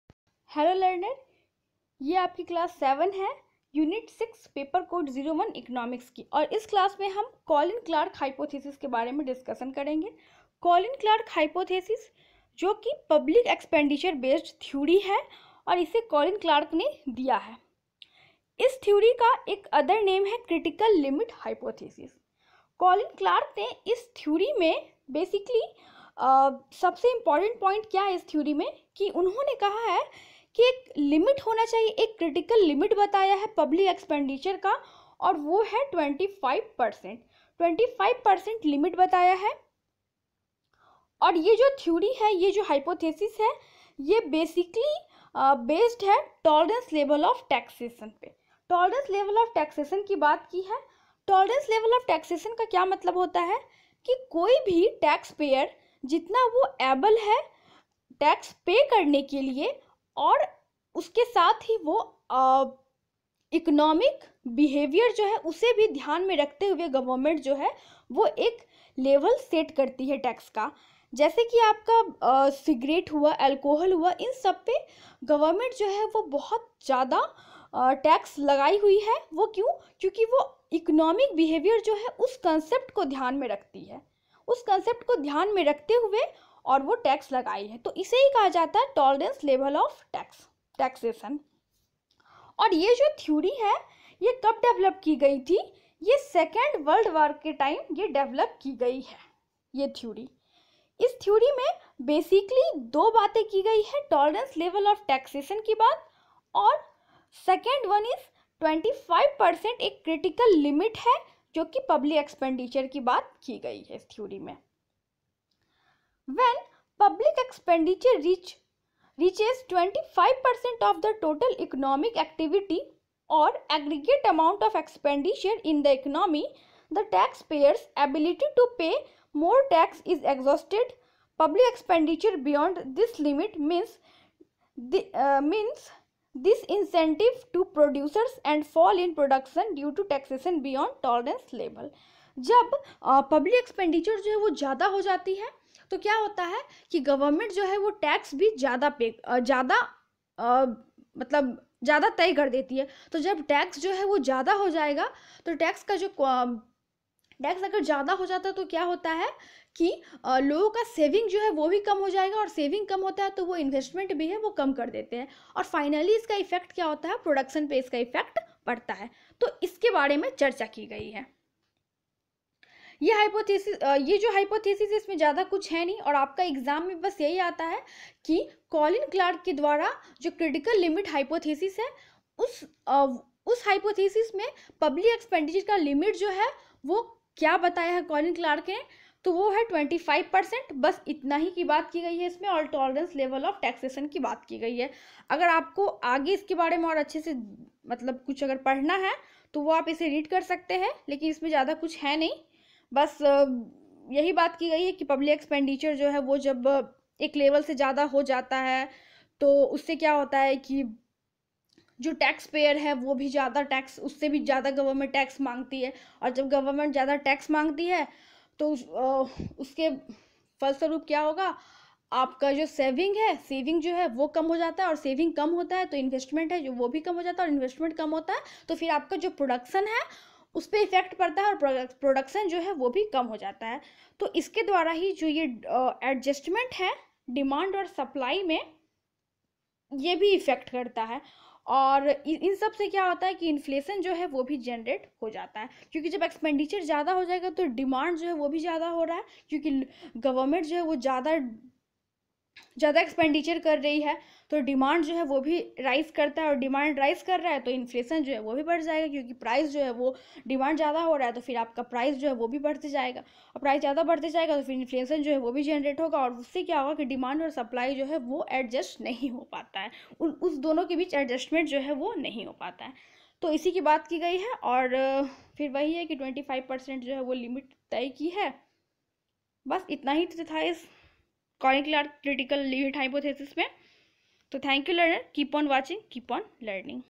Colin Clark Hypothesis Unit-6 Class-7 | Economics | Paper Code- 1
Monika Ma'am is teaching about Colin Clark Hypothesis in this lecture.
For downloading notes in PDF format please visit my Digital Store
Keep on watching, keep on learning!